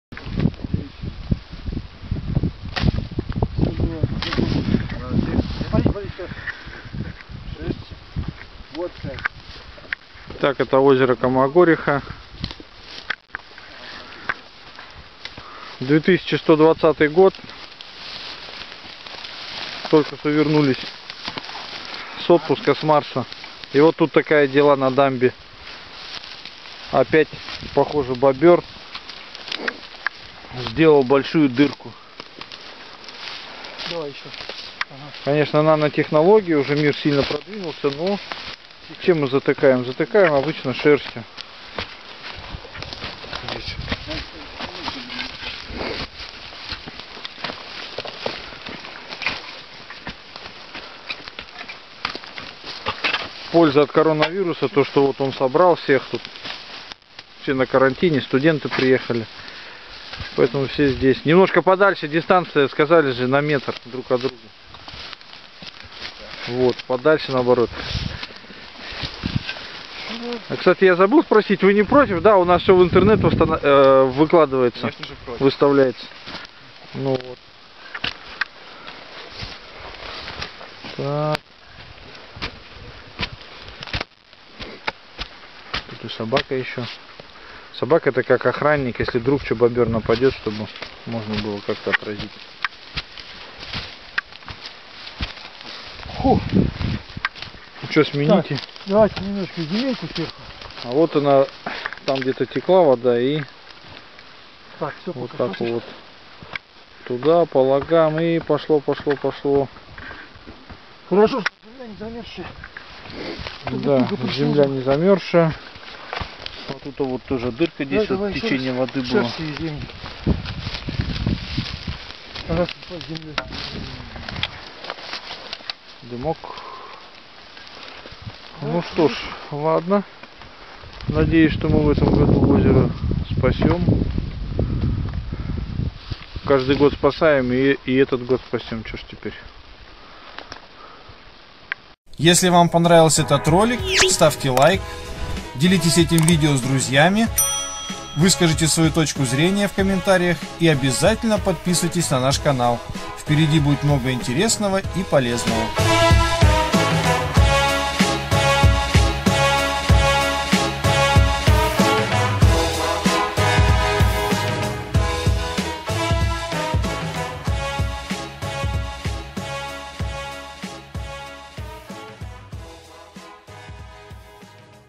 Бобер прогрыз дамбу. Деревенские вредители в Потеряевке!!!
И снова бобер! На нашем главном и самом перспективном озере была обнаружена течь в дамбе и если во время ничего не предпринять, то всё озеро может убежать в лог на другую сторону дамбы. Группа быстрого реагирования с задачей справились, позже когда была обнаружена течь в другом месте - устранили и её. Конечно "заплатки" не решают устранения  данного вопроса, здесь нужен серьезный, ответственный и затратный ПОДХОД. Но как гласит легенда - лепили из того что было, а дальше будь что будет...)))